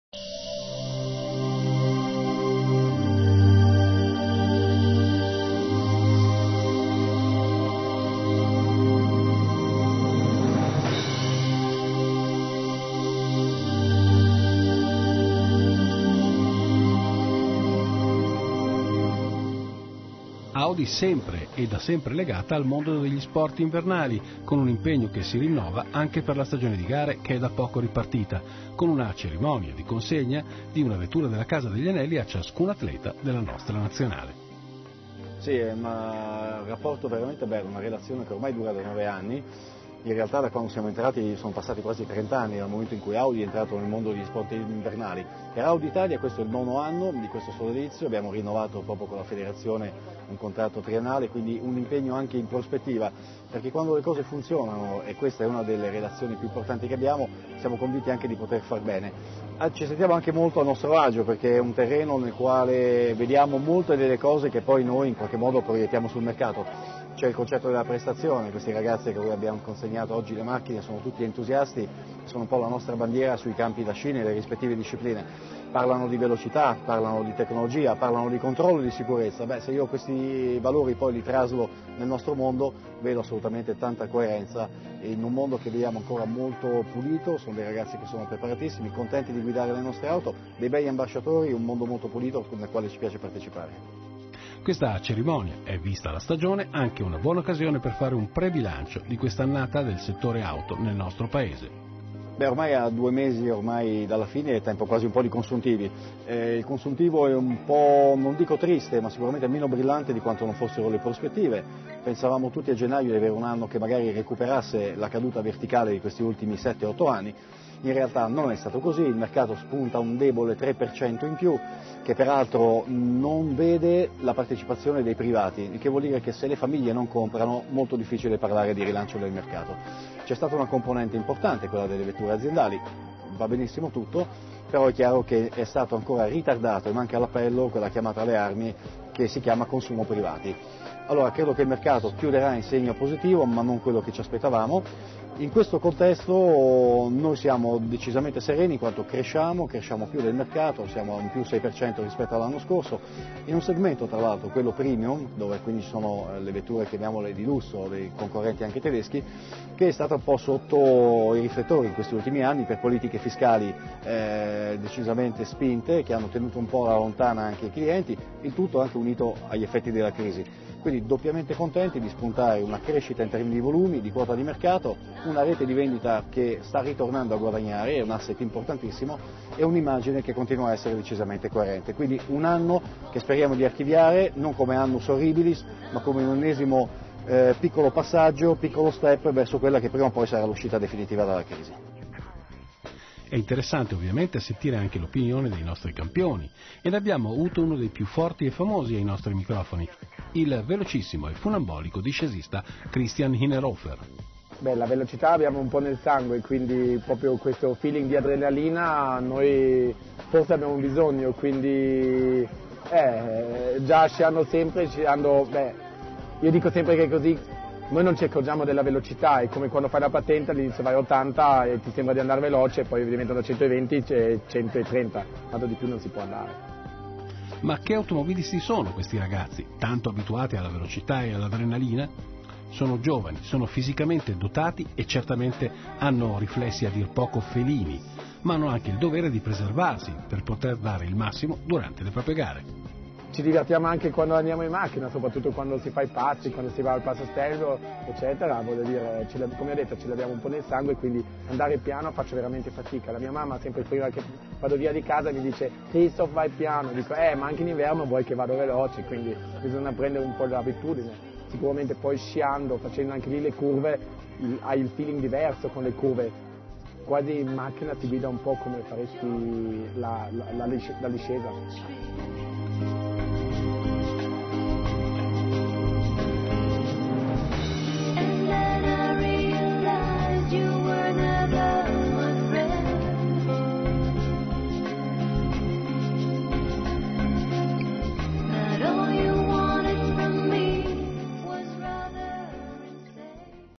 Class Ride&Drive - Audi FISI
Bellissimo servizio con la partecipazioone straordinaria della Federazione Italiana Sport Invernali